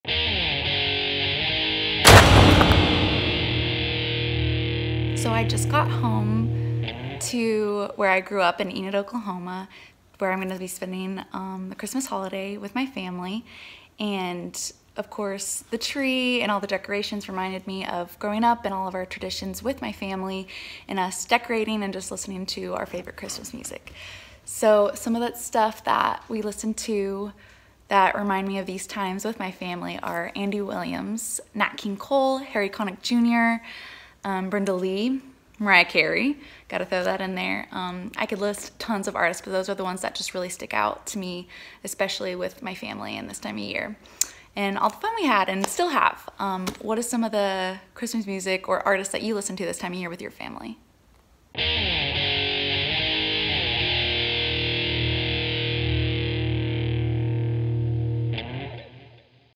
Holiday Music with the family
Emily tells about listening to Holiday music with the Fam.